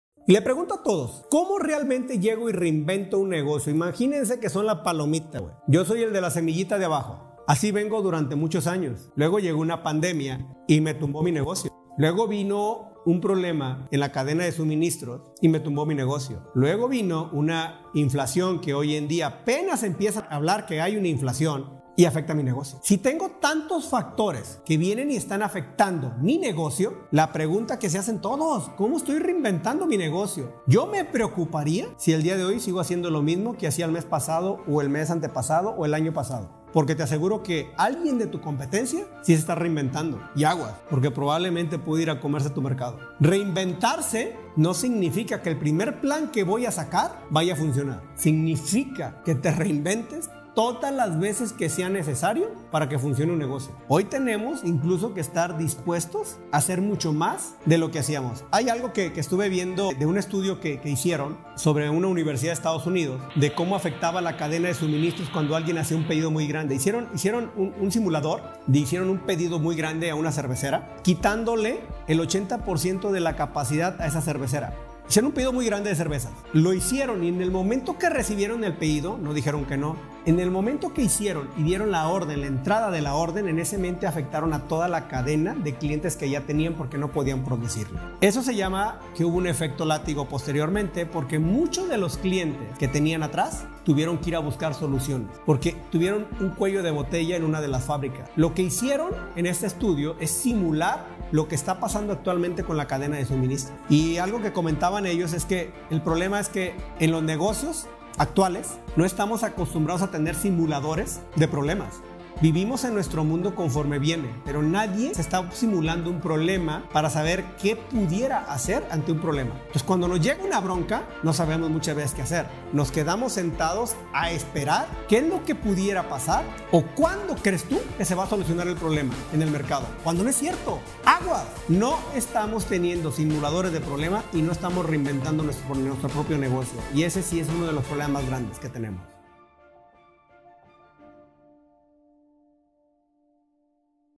Reinventa tu negocio
Si seguimos haciendo lo mismo que hace seis meses o hace tres o hace un mes… es momento de parar, analizar y ver si te está funcionado esa estrategia y lo más seguro es hoy en día sea obsoleto lo que haces o no te esté funcionando de la misma manera y es ahí donde debemos de cambiar, de reinventar el negocio.

Reinventarse puede cansado y frustrante porque tendrás que reinventarte todas las veces que sea necesario. Señores, hoy en día no hay nada establecido y probablemente tengas que crear nuevas estrategias constantemente, pero tus resultados serán mucho mejores.